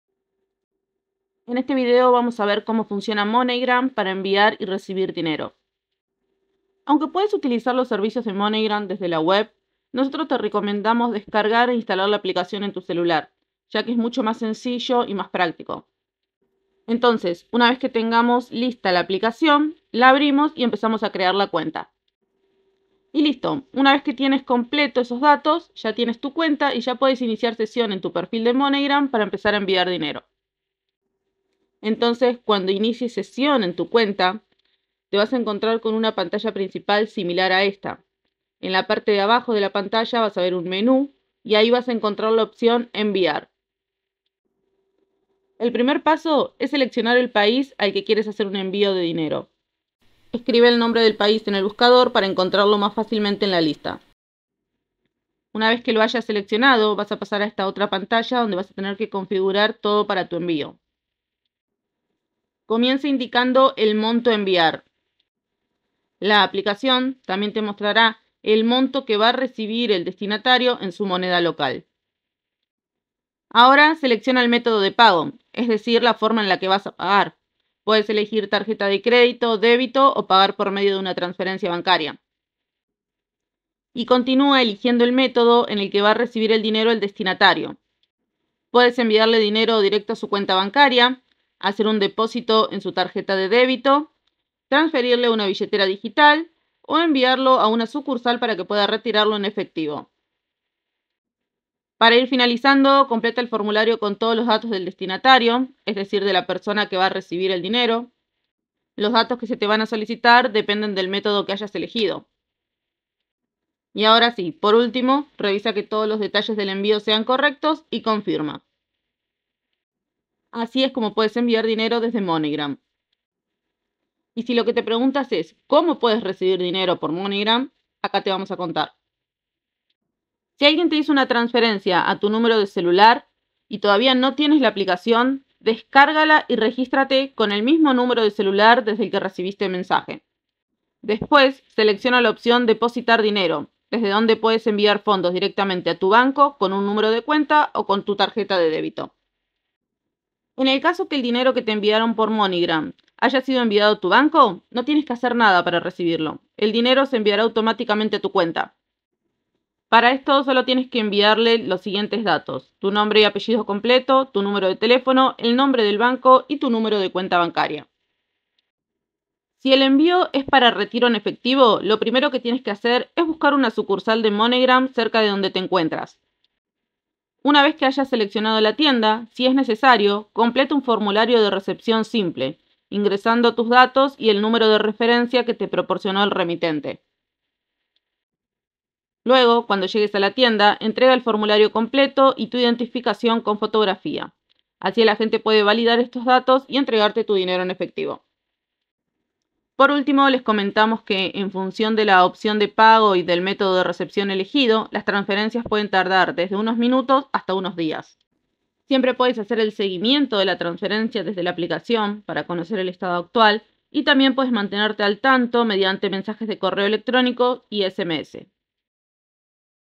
MoneyGram: Como FUNCIONA? Como ENVIAR y RECIBIR Dinero? - Tutorial Completo! (2024)
✅ Descubre que es moneygram y como funciona moneygram para enviar y recibir dinero y para poder hacer transferencias en moneygram en este tutorial completo.

Comparte tu experiencia en los comentarios de abajo si esto funcionó para ti. Apreciamos todo tu apoyo!